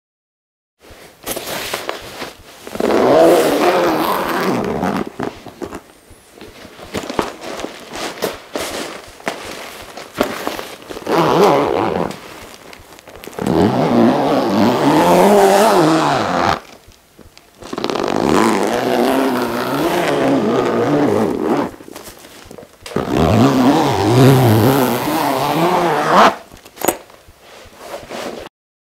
Pulling a Zipper of a Travel or Sports Bag Sound Effect | SFX Free for Non-Profit Projects
Pulling the zipper of a travel bag or a sports bag sound effect recording. This SFX sound is free for your non-profit project.